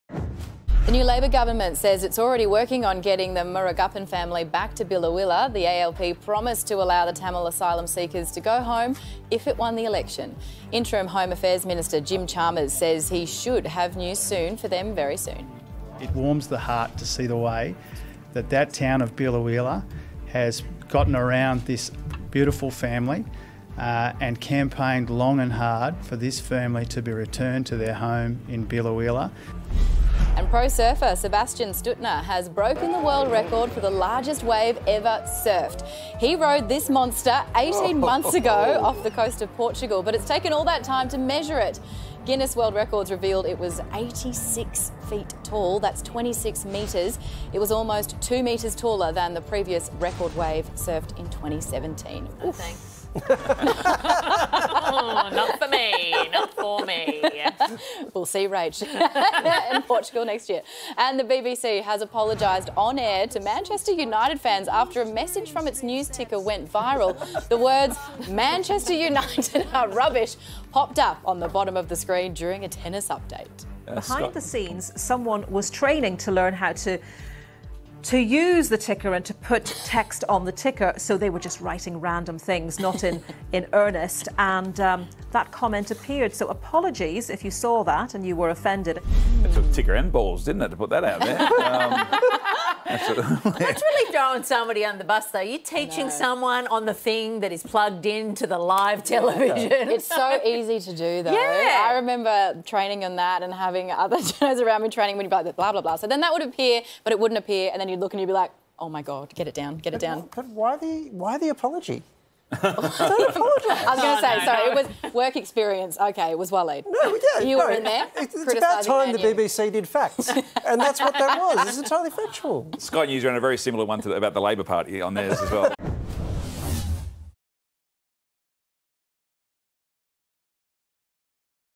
Jim Chalmers Says 'Substantial Progress' Has Been Made To Return Murugappan Family To Biloela
Interim Home Affairs Minister Jim Chalmers has said 'substantial progress' has been made to return the Murugappan family home to Biloela.